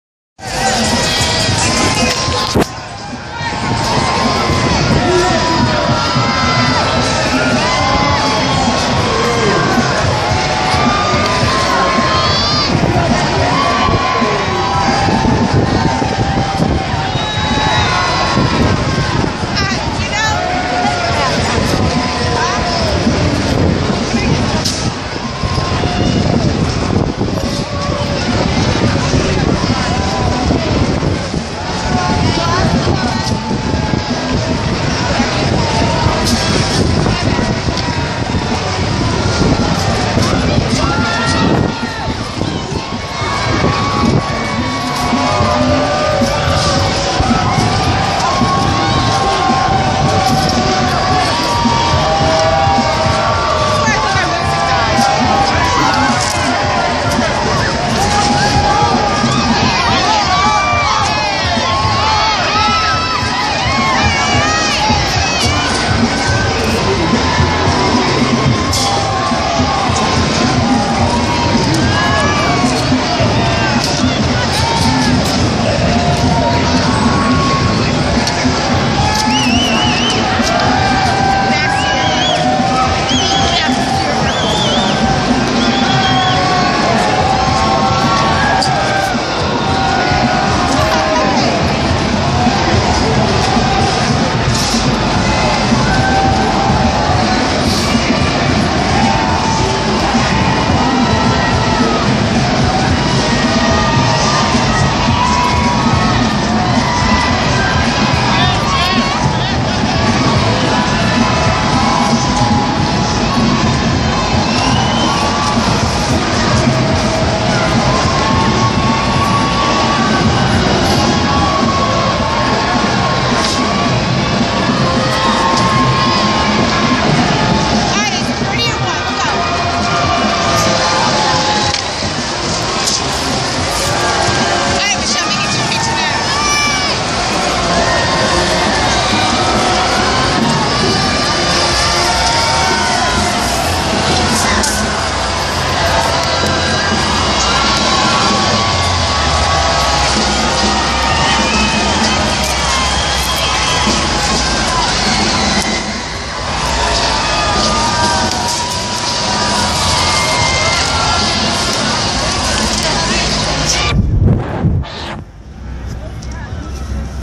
Krewe of Cleopatra, parade in NOLA, Jan. 29, 2016.
Arrived in The French Quarter at Canal Street in New Orleans atop float number 14, "YMCA"; Cleopatra  Rocks The Big Easy," January 29, 2016.